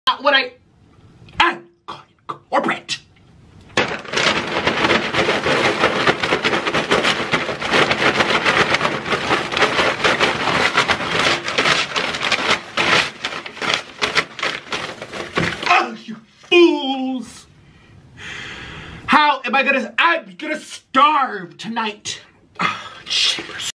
Nickado Avocado having a meltdown for literally no reason
Nick just melting down for no apparent reason. What can u expect from such a skinny legend.